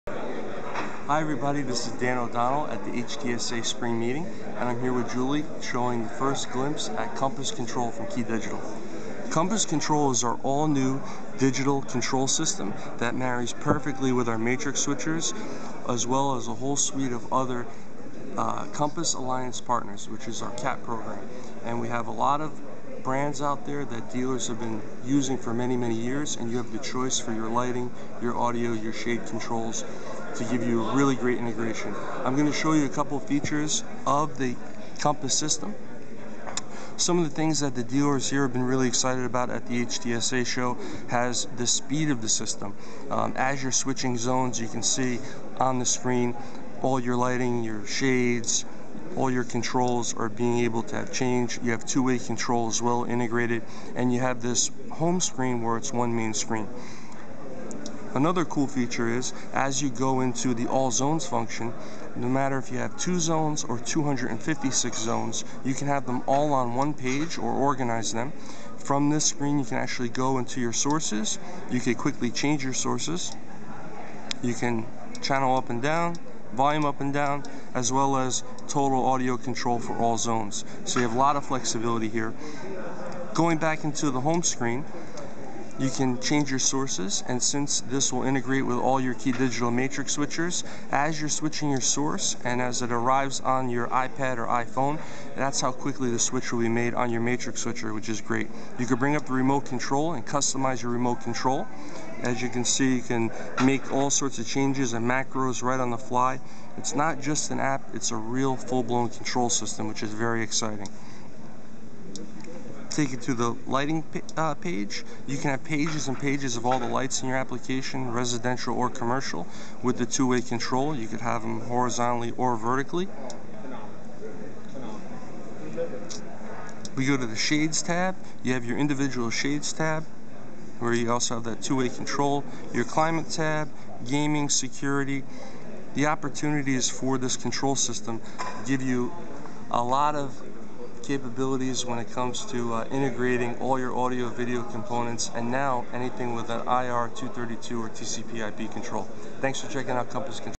Hands On: Key Digital Compass Control Home Automation System
Key Digital's national sales manager Dan O'Donnell details the new Compass Control system during the HTSA spring meeting.